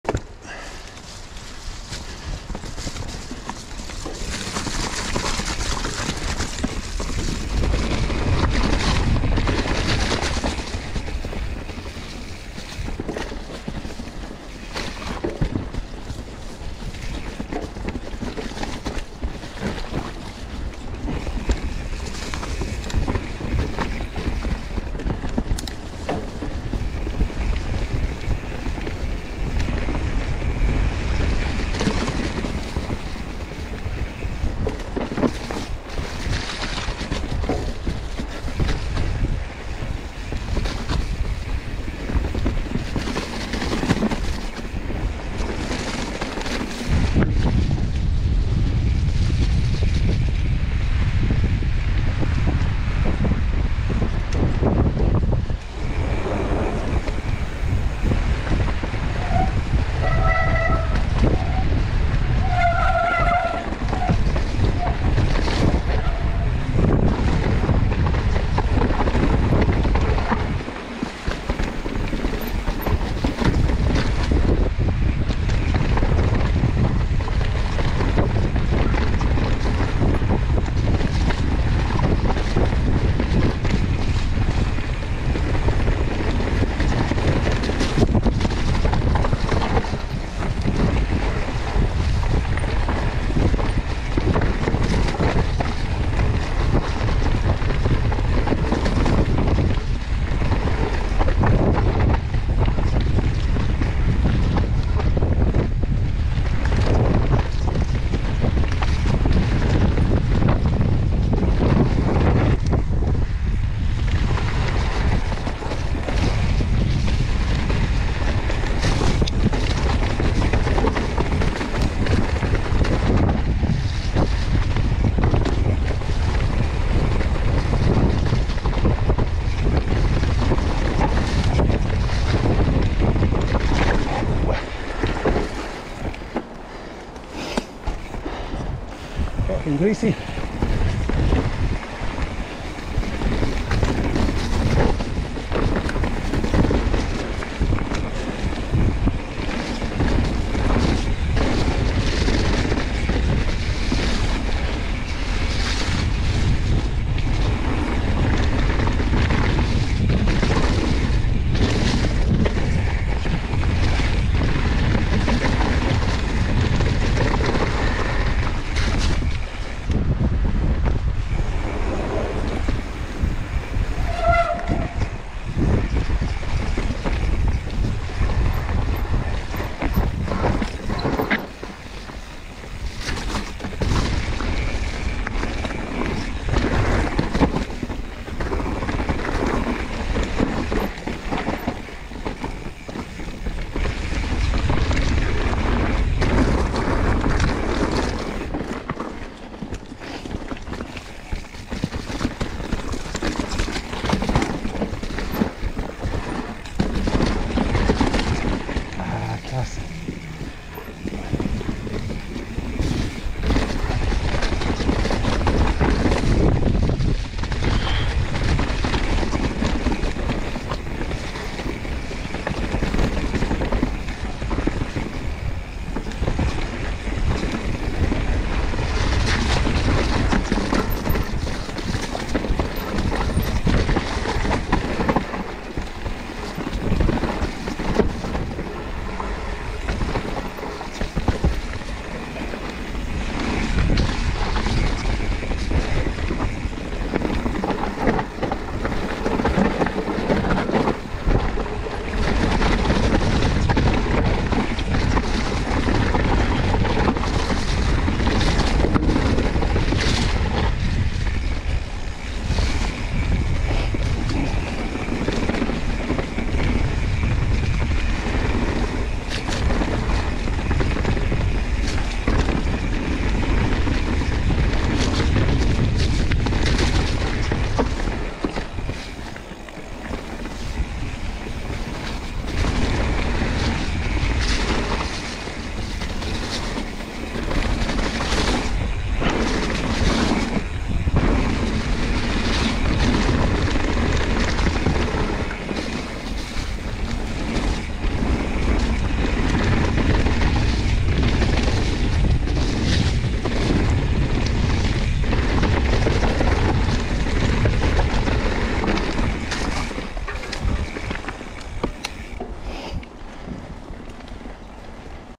HUSH MTB-Sasquatch and Straight Sideways
These trails start from the same point but branch off down the trail. All bigger features, such as the drop on sasquatch, can be avoided. Like all HUSH trails, these are well built and not too difficult, although it was very wet and greasy on this particular day with a few slips and slides!